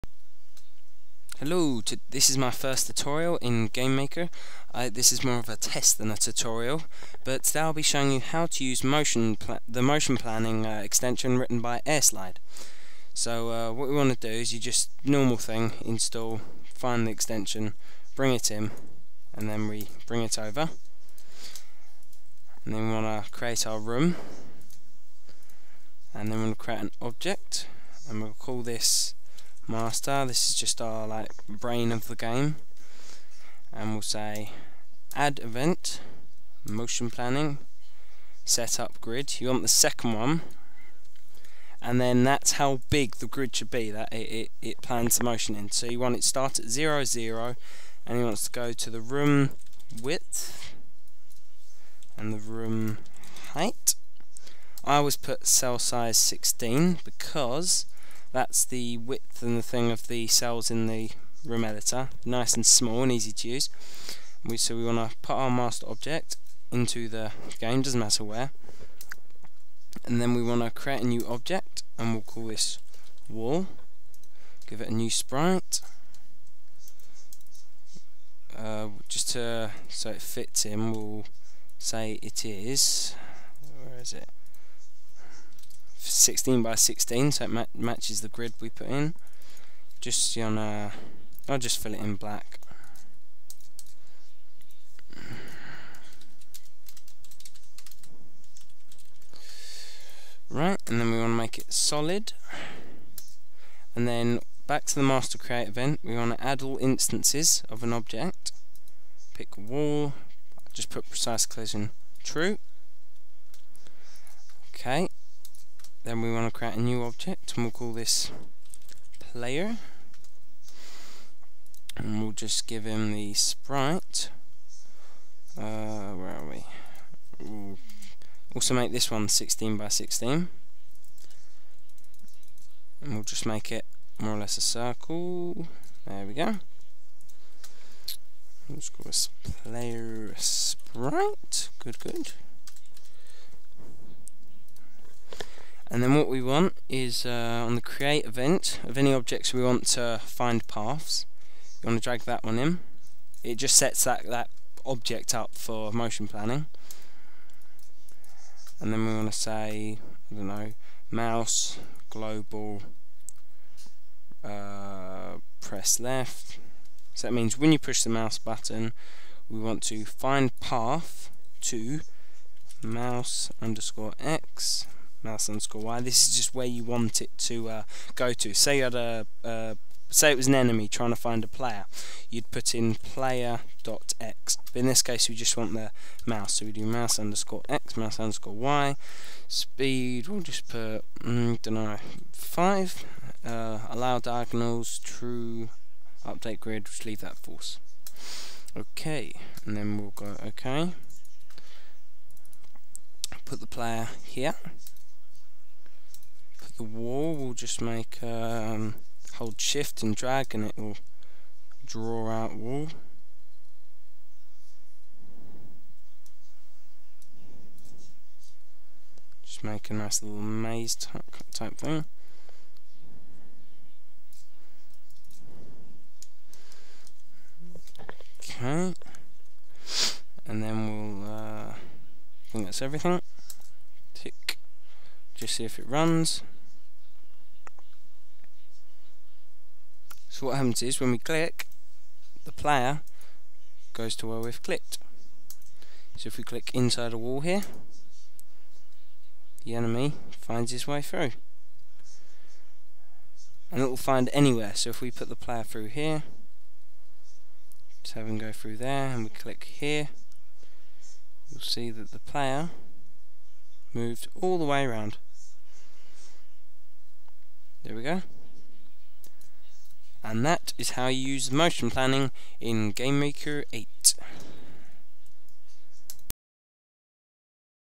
Game Maker: Motion Planning/Pathfinding
This is the first video of a series showing how to use Game Maker, in this video I show you how to perform Pathfinding in game maker using an extension made by Airslide, link to his extension will appear here soon, the game maker community is offline for a few days while they upgrade their site :-)